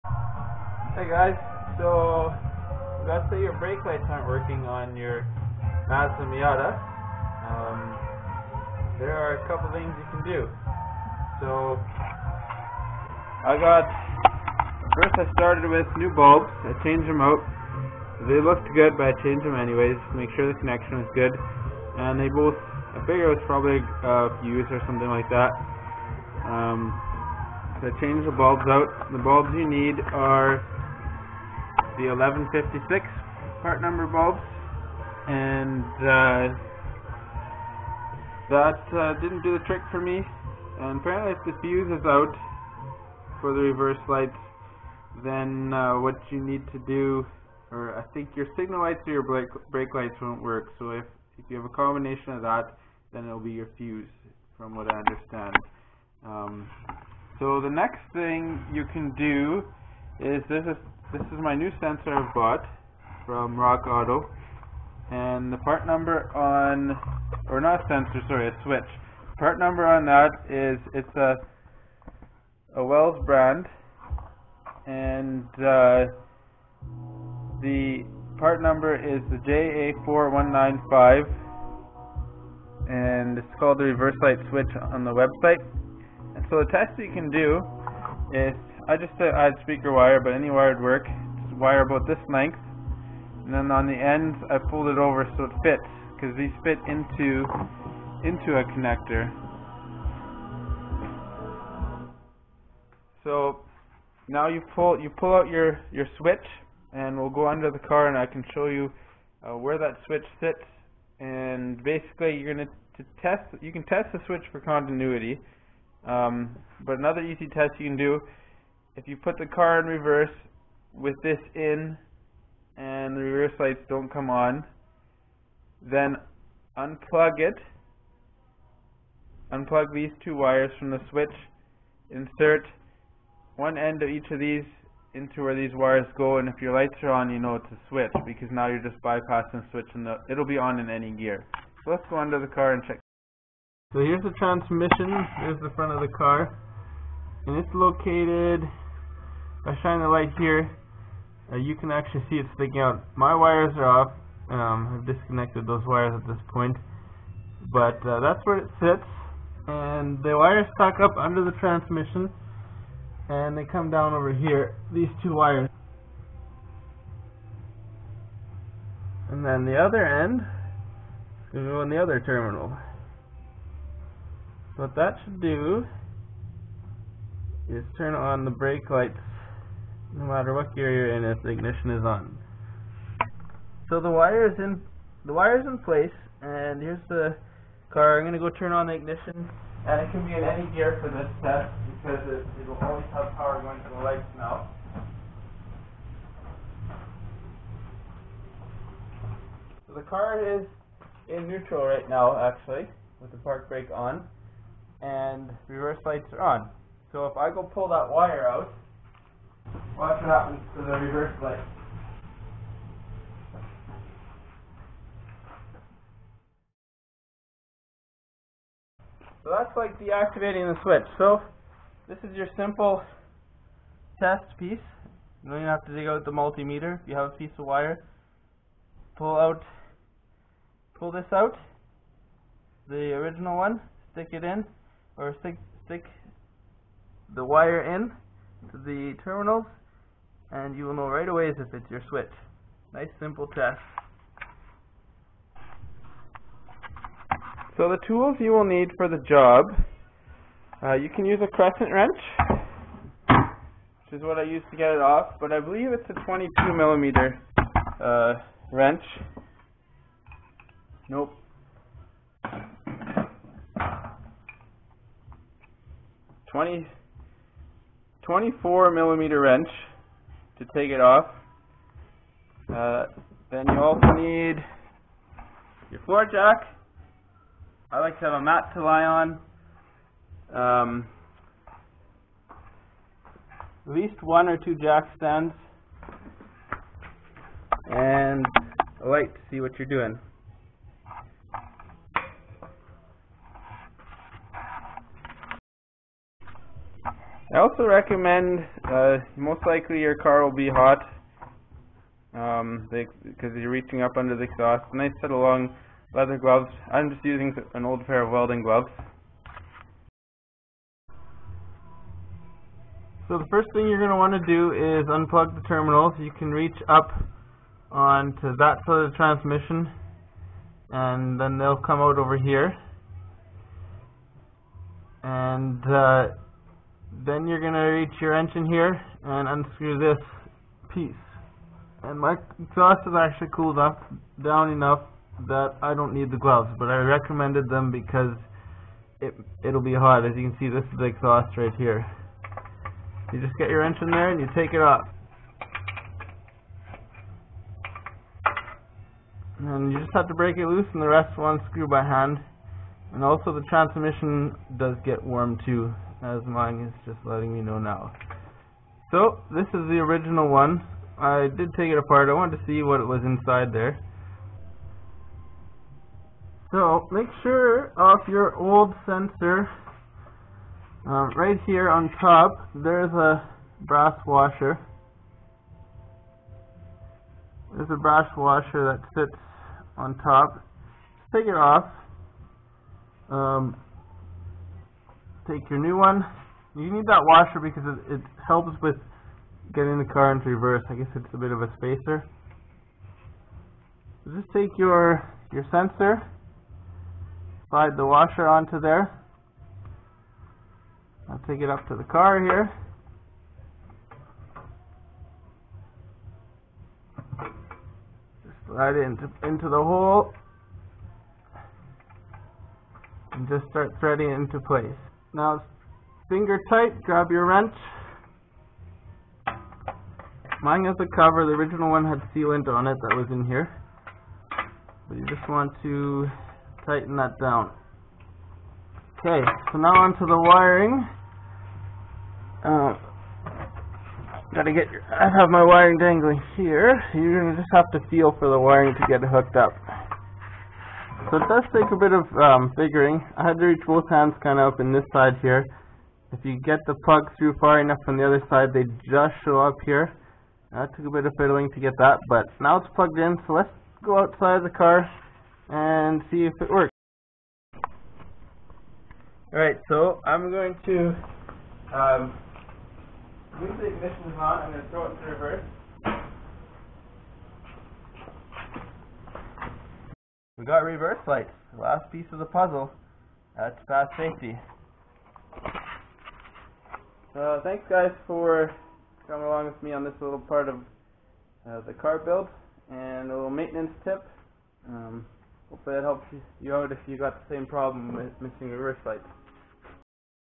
Miata reverse light fix!
How to repair your broken reverse lights in an na miata.